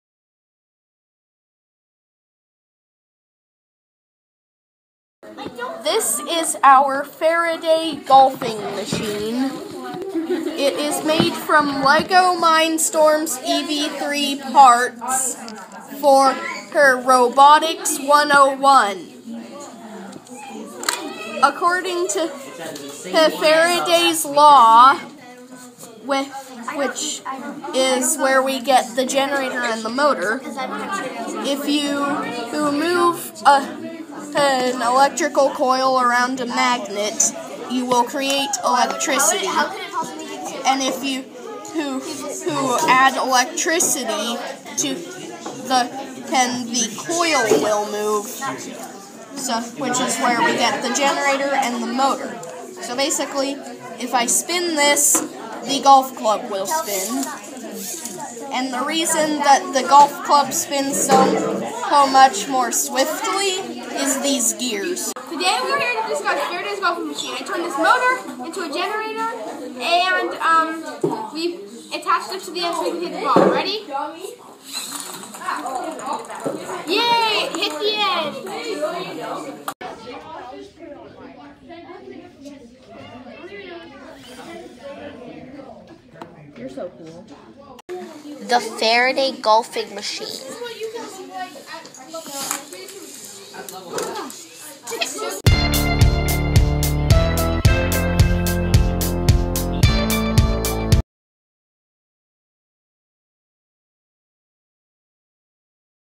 Faraday Golfing Machine
Video of students showing off their Faraday Golfing Machine STEM Robotics 101 Challenge final products. Students learn about Farday's Law and his electromagnetic experiments that led to the invention of the electric motor and the generator. Using Faraday's Law students design golfing machines using the Lego Mindstorms EV3 motors.